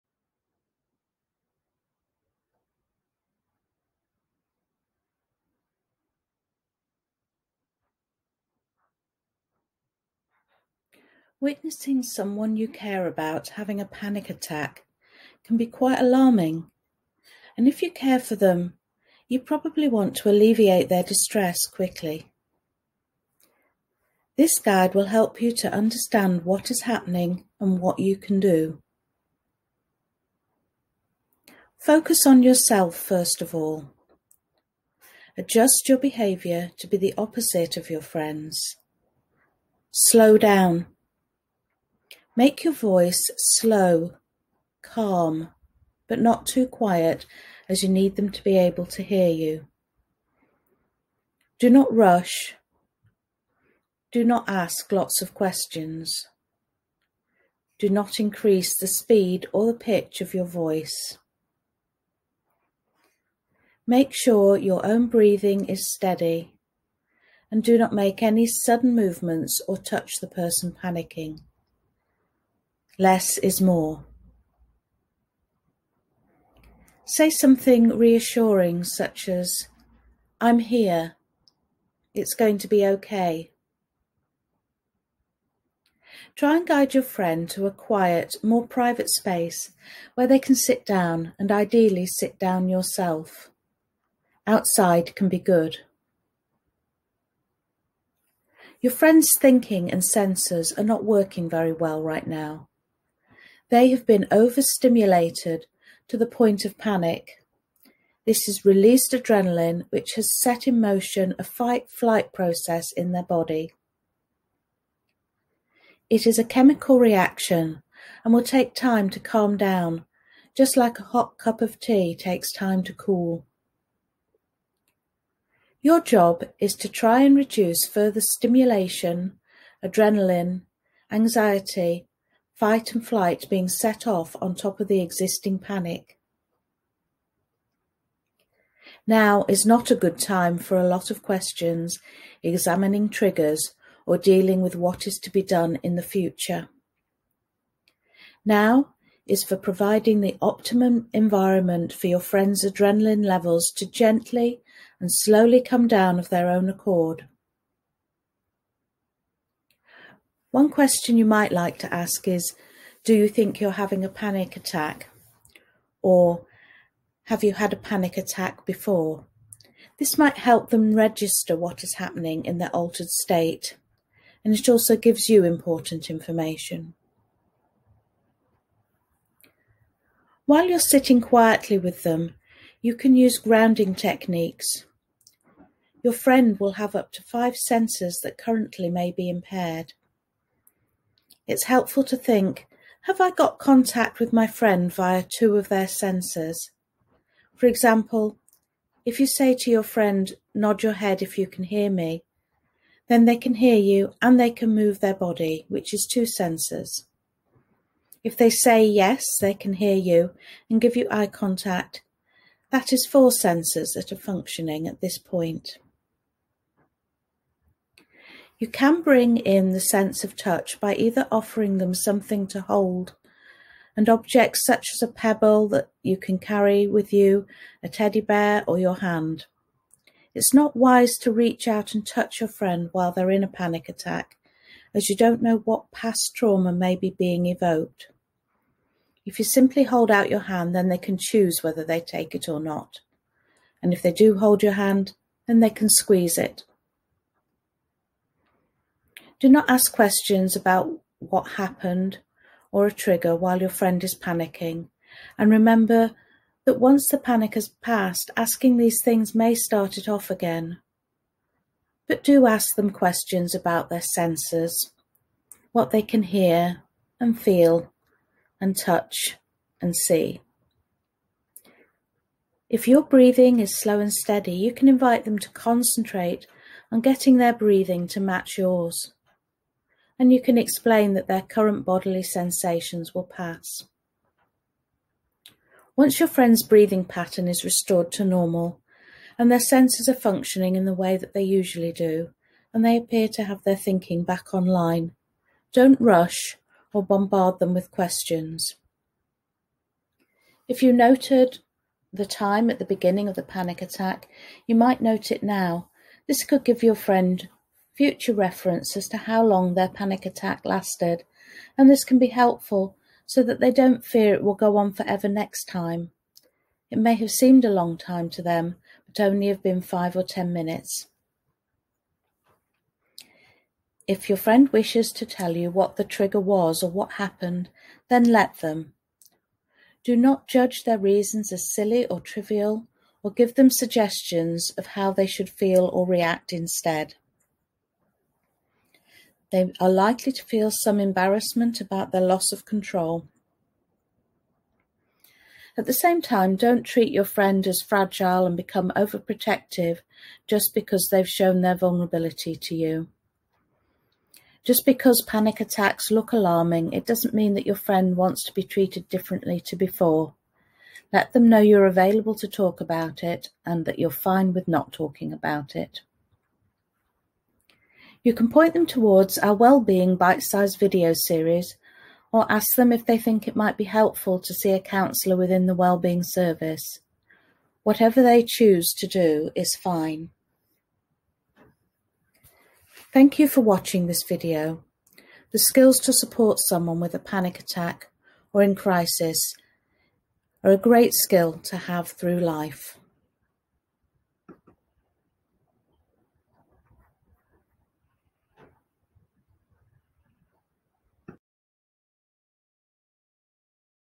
Supporting someone through panic
A short video from the Student Wellbeing Service at Sheffield Hallam University offering advice for supporting someone through panic.